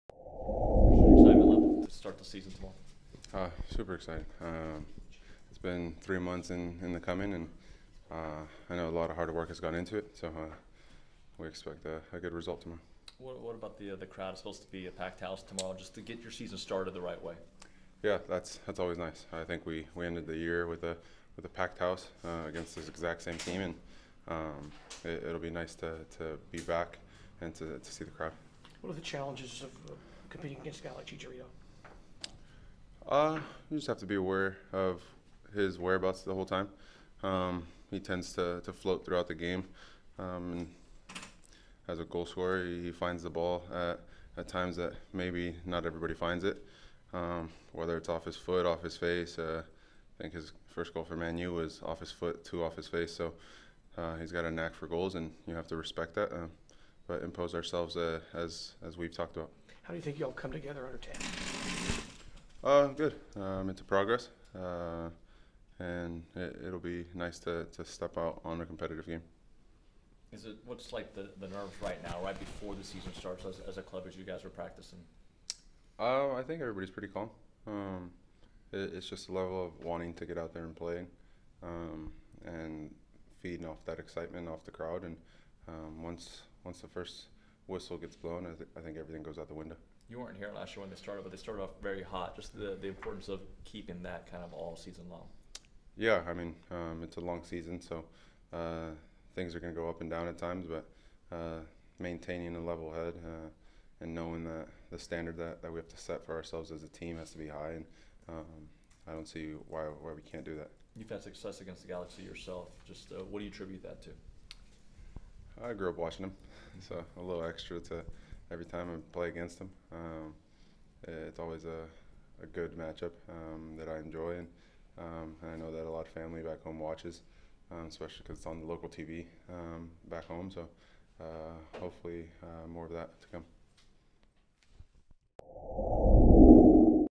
Christian Ramirez 2.28.2020
Christian Ramirez on the start of the 2020 season and playing the LA Galaxy